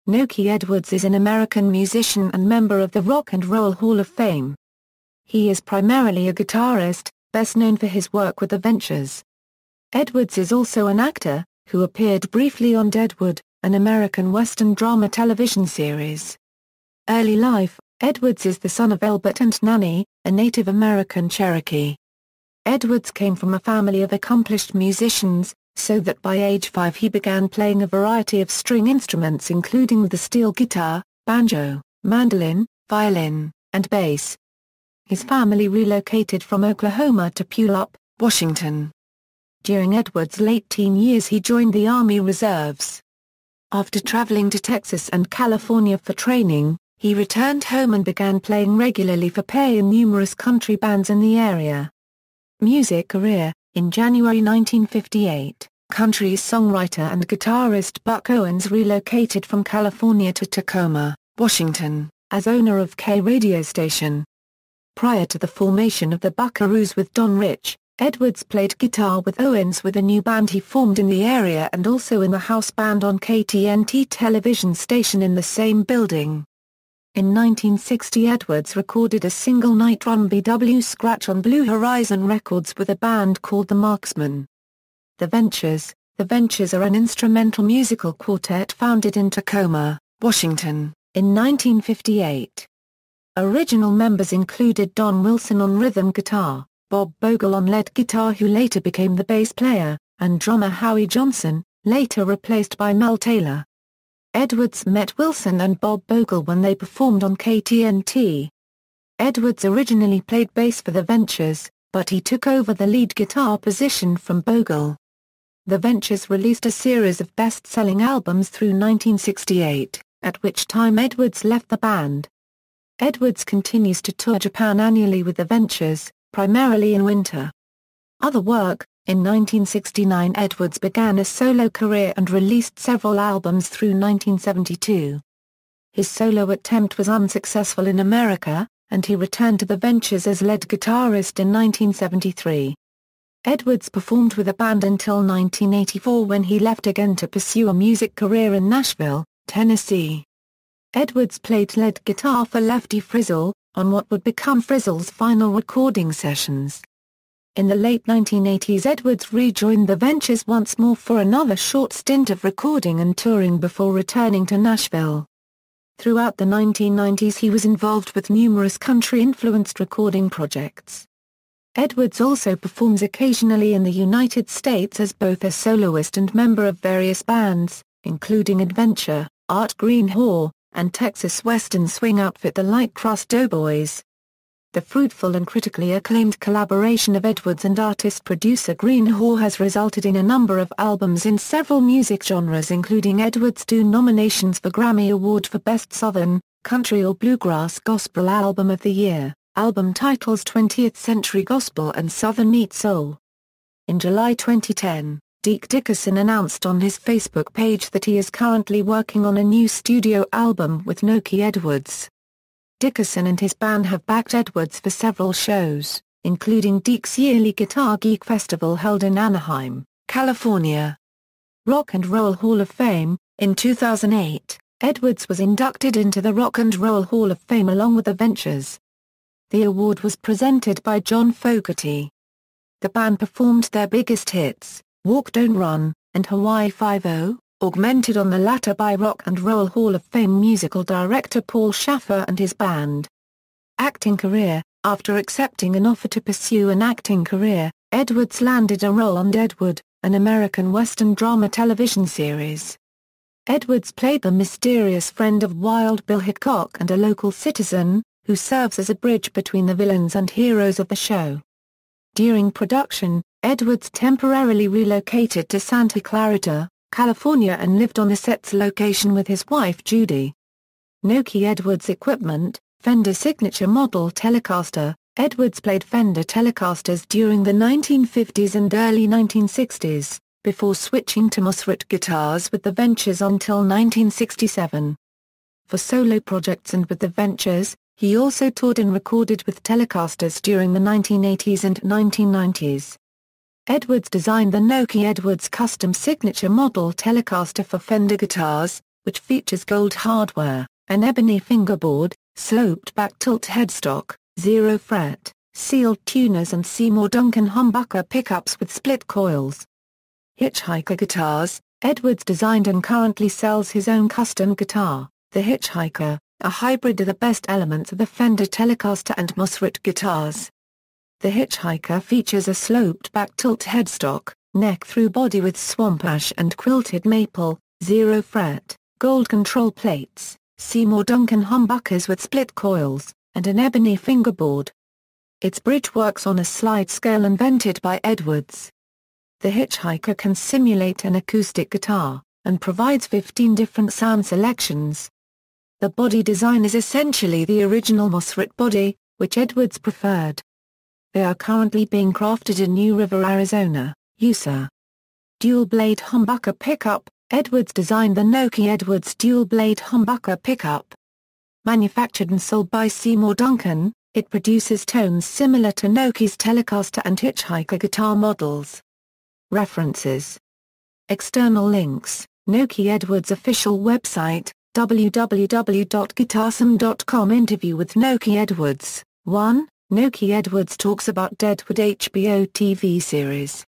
Nokie Edwards
Nokie Edwards (born Nole Floyd Edwards, May 9, 1935, Lahoma, Oklahoma) is an American musician and member of The Rock and Roll Hall of Fame. He is primarily a guitarist, best known for his work with The Ventures. Edwards is also an actor, who appeared briefly on Deadwood, an American Western drama television series.